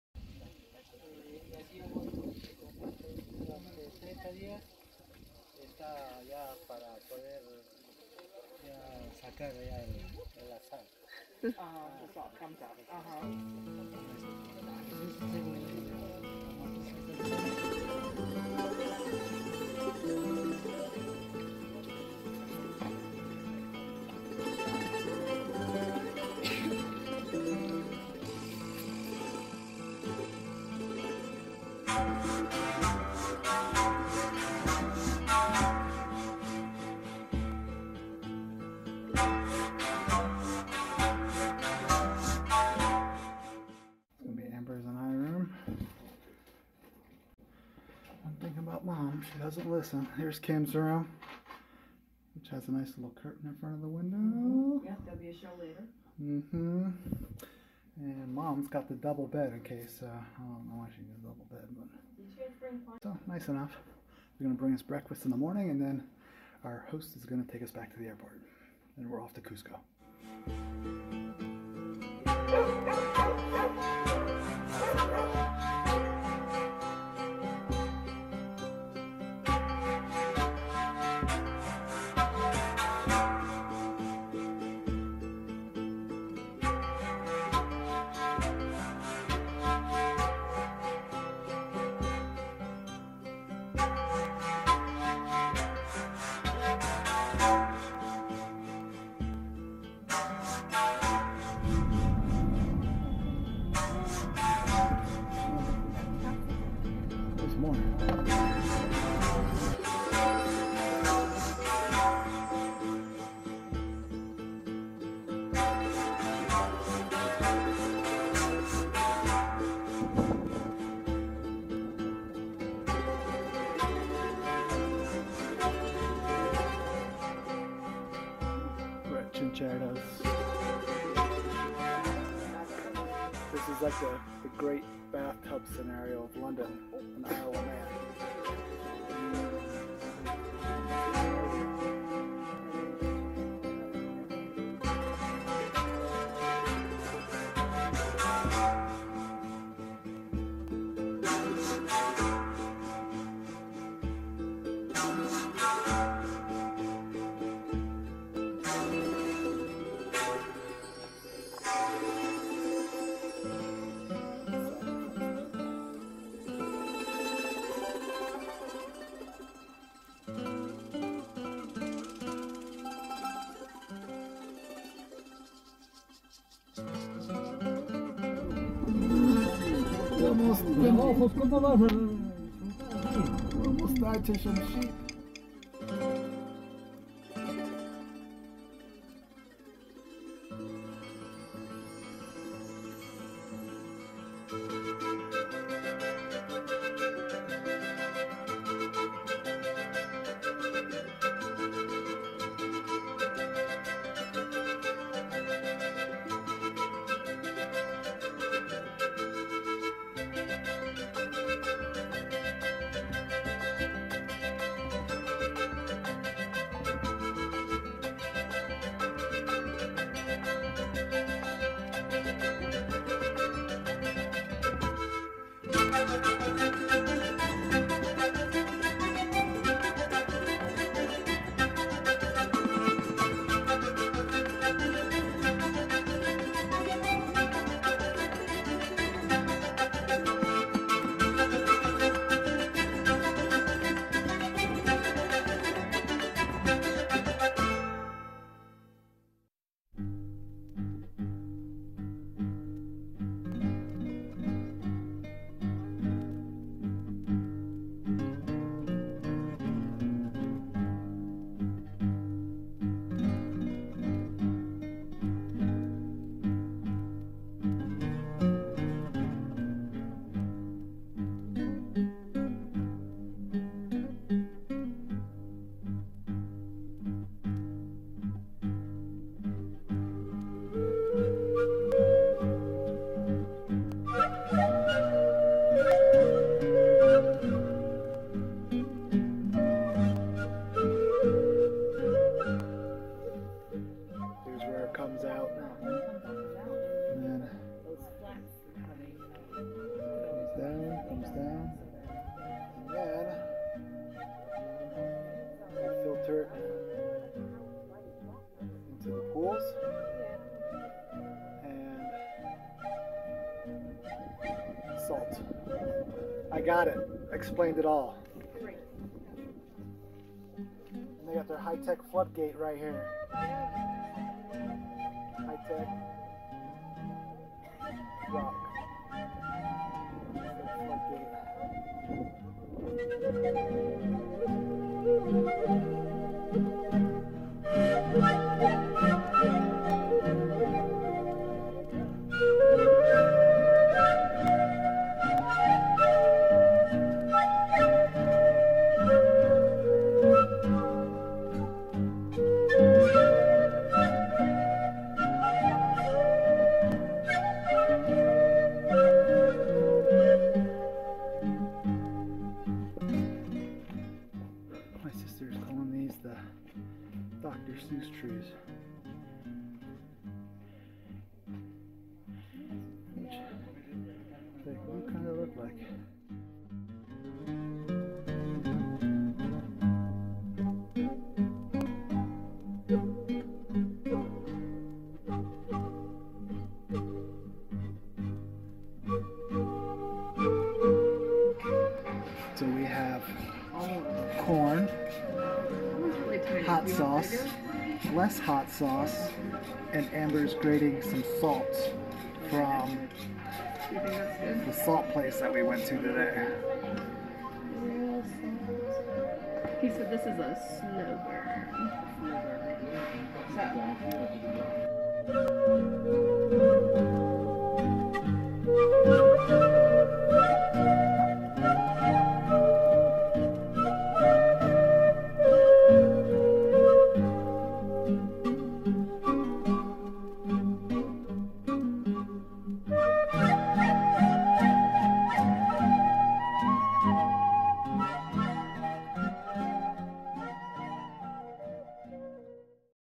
Peru - Sacred Valley - Sept 2018
September 19th, 2018, Amber (wife), Kim (Sister), mom and I ventured to Peru for major buck list checkoff to Machu Picchu!  Flying into Lima Peru we landed close to midnight.  After a few hours sleep, back to airport and off to Cusco.  Our AirBNB in  Ollantaytambo had a driver pick us up and on our way threw the Sacred Valley visited: Chinchero, Moray and Maras Salt Mines.  Wrapped up our day with a nice meal at Chuncho on the city square.

Licensed to YouTube by    The Orchard Music (on behalf of Discolandia Dueri & Cia. Ltda.)